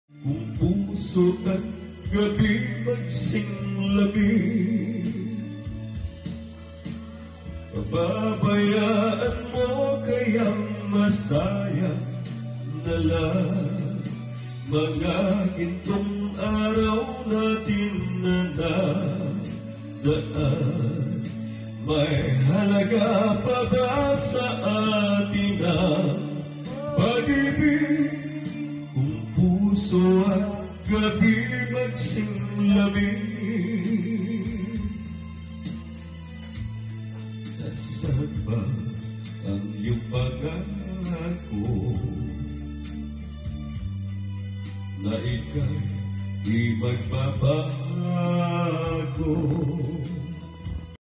Kanta muna
Pakinggan natin si kuya ang ganda ng boses